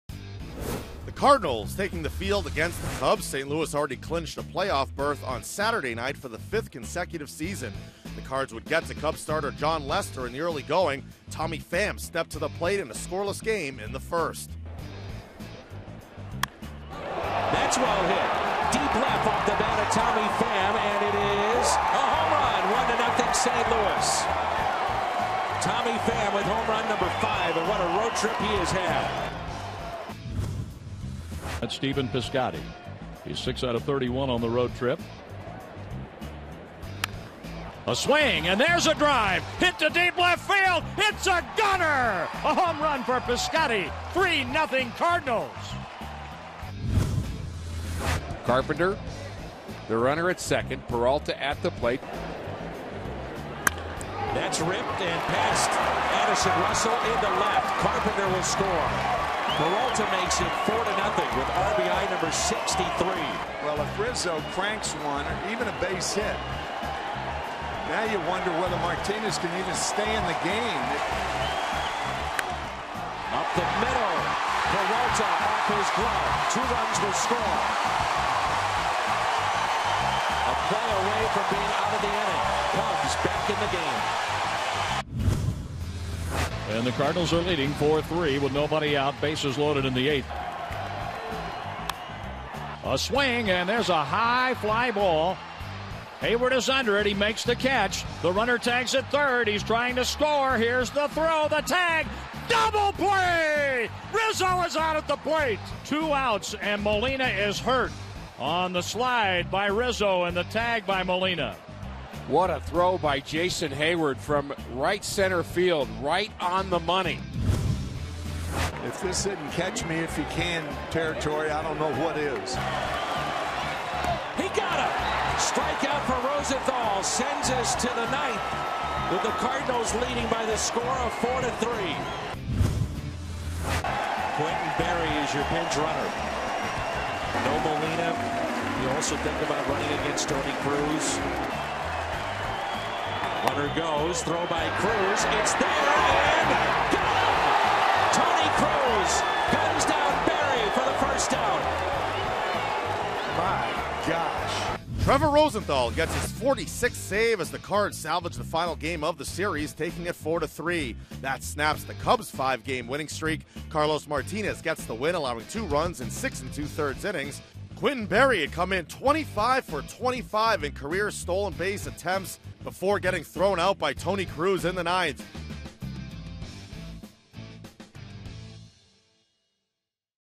9/20/15: Early runs lift Cardinals in series finale
Daily Recap: Early offense against Jon Lester was enough for Carlos Martinez and the Cardinals in a series finale nail-biter